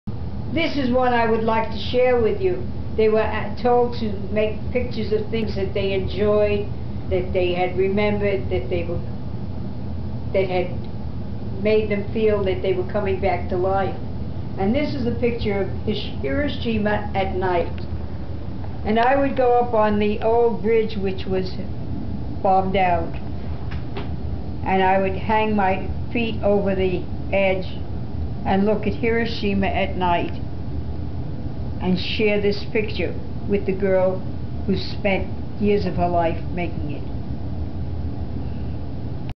Phyllis Rodin on Hiroshima at Night, 4th of 8 videos
Kiri-e, fabric collage pictures by survivors of Hiroshima's aftermath. An exhibition at the C.X.Silver Gallery in West Brattleboro of exquisite paintings using tiny pieces of colored cloth as the medium, done by student survivors of the 1945 bombing who were rescued by a teacher and taught this traditional painting technique to restore a sense of normalcy to their lives. The teacher, Atika-san, instructed the students to paint some thing or place or person of beauty as a way of attempting to transcend inner and outer chaos. The subject matter of the paintings vary from beloved dolls, to tranquil courtyards and countryside, fond memories of festivals, potted flowers and bonsai, to classic pictures of women reminding us of Utamaro's wood block prints. One depicted Hiroshima at night in an era of peace. Another depicted the remains of the bridge on which many of her peers perished. Before they painted, they had to recuperate enough to go out and salvage whatever colored cloth they could find in the ashes and debris. They brought the cloth back, cleaned it of soot, and then followed Ataki-san's direction to create beautiful compositions of subjects of beauty.

The collection of kiri-e belongs to Phyllis Rodin of Amherst, MA, who has turned 99 this year (2013). Phyllis was given this collection in the 1960s by Atika-san on behalf of the city of Hiroshima in appreciation for Phyllis serving the people of Hiroshima in the hospital there for one and a half years in the mid sixties. Phyllis was there helping any way she could, listening and spending time with patients, helping them deal with their flashbacks from twenty years earlier.  With this precious collection of kiri-e, Phyllis has toured many cities and countries raising awareness of the dangers of nuclear devastation and war and the alternative of Peace through Beauty.  The collection is available for exhibition and other venues continue to be actively sought. Phyllis has been an inspiring speaker about her cause for Creating Peace with a determination to keep reaching out to others.

2. The gallery is located at 814 Western Avenue, West Brattleboro, Vermont on Vermont Route 9, one mile west of Interstate 91, Exit 2, on the left. 1, . More information on the kiri-e is Further information on Asian Cultural Center of Vermont can be found by exploring the accvt.org website.